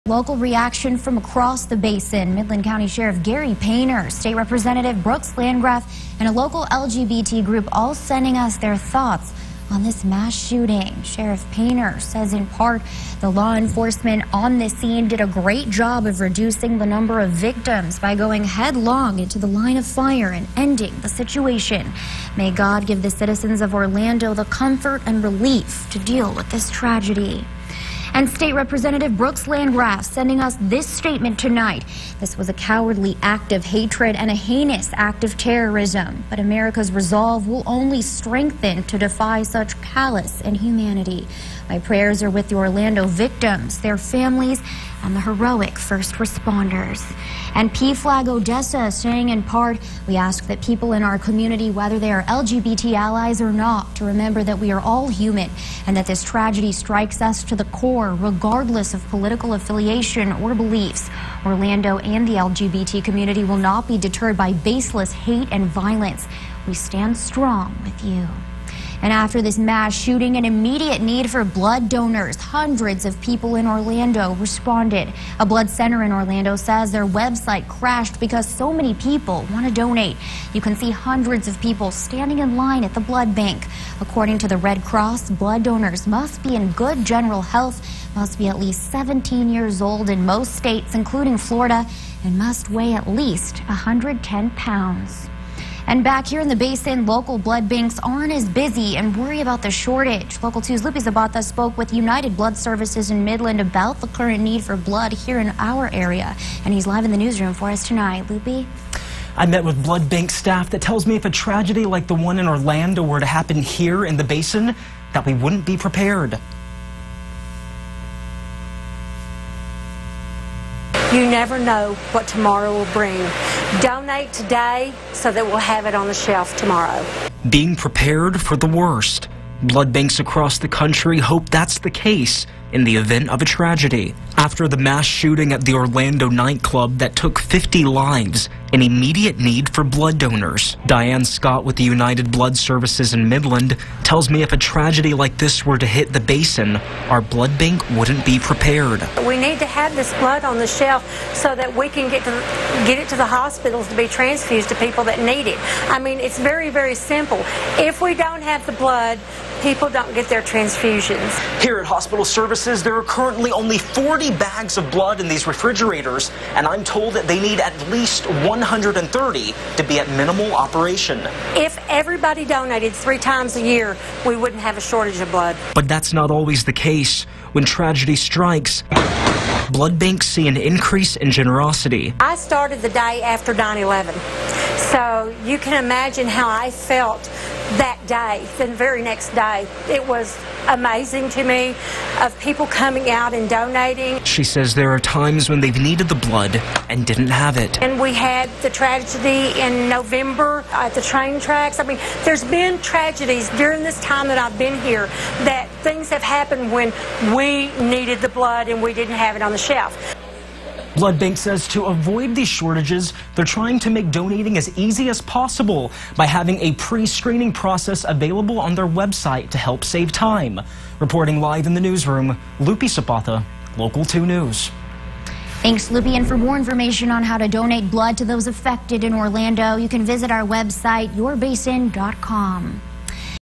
Midland Blood Bank Appeals For Critical Donations
United Blood Services in Midland has issued a Critical Appeal for blood. Blood bank staff says if a tragedy like the one in Orlando were to happened in the Basin, they wouldn't be prepared.